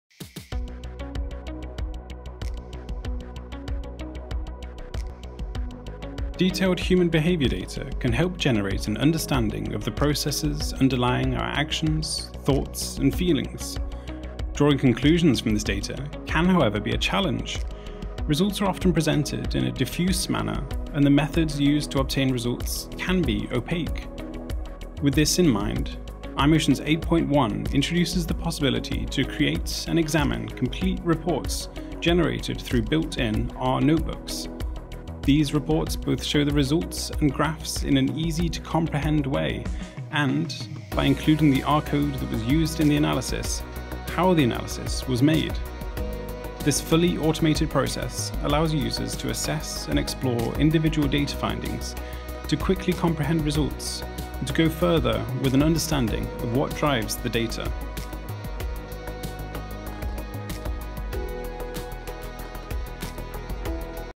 iMotions 8.1 - Data Reports from R Notebooks
The R integration now also allows detailed participant reports to be easily generated. Transparent methods and results can be shown on an individual level:

Method descriptions – the methods used to generate the results are explained in accessible terms, streamlining the process of article submission.
Fully transparent – all R code that is used to generate the results is shown, allowing you to understand the methodology in detail. References for the methods are also provided.
Individualized graphs – a detailed understanding of the signals can be generated from the automatically generated graphs.